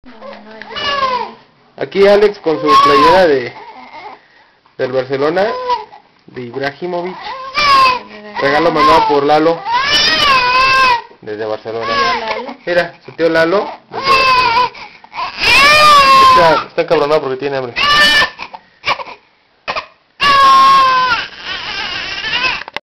Alex con su playerita de Ibrahimovic
Alex con su playerita del Barcelona con el 9 de Ibrahimovic regalazo de su tio LALO